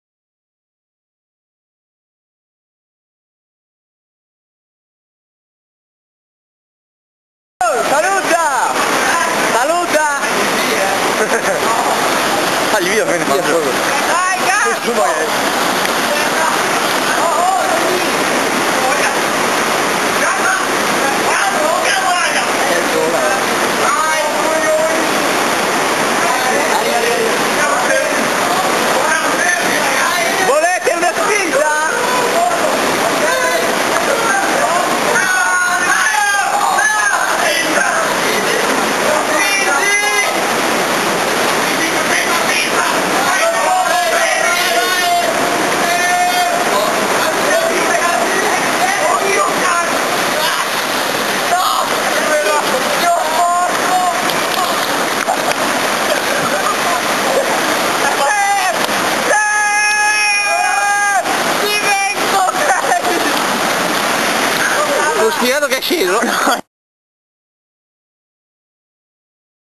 acqualandia al piave
noi, al piave di soverzene ci divertiamo con il nostro nuovo gommone...